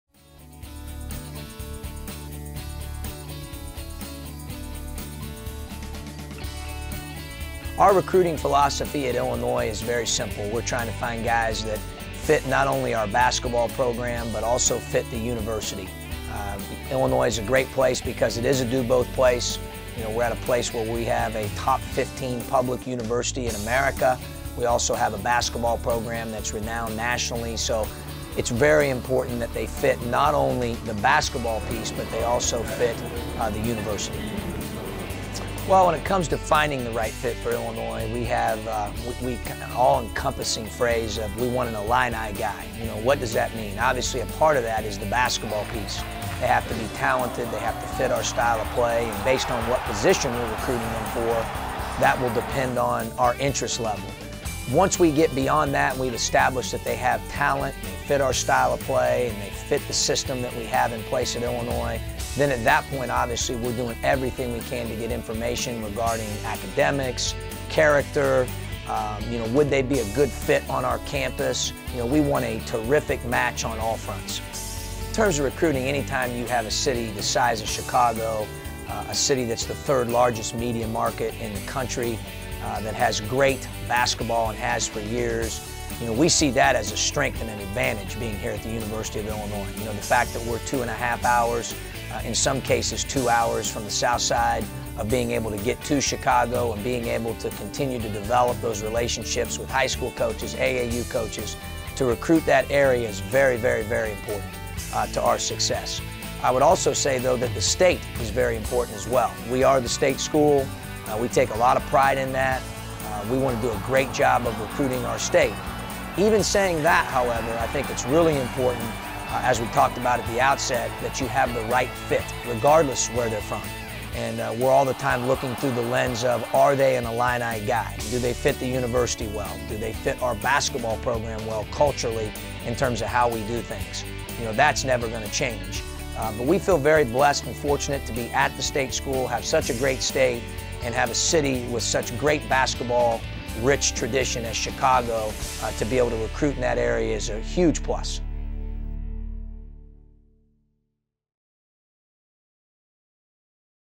John Groce One-On-One - Recruiting Philosophy
Illinois basketball coach John Groce talks about his recruiting philosophy - July 2013